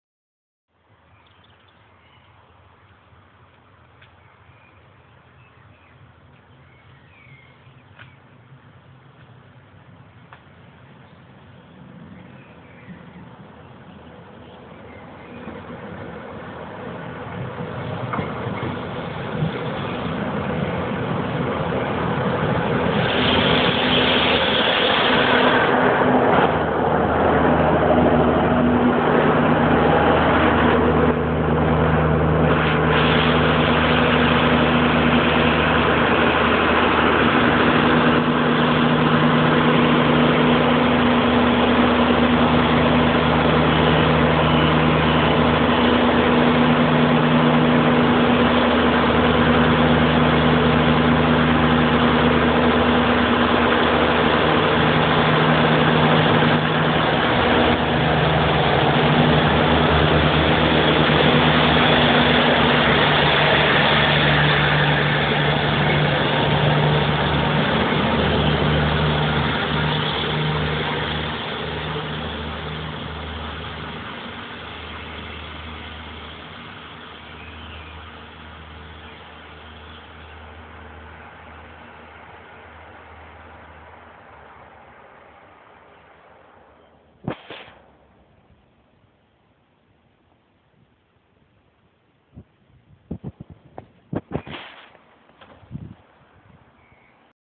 Quiet evening in Castleconnell
The evening Ballybrophy-Limerick train pauses at Castleconnell on 10/5/2011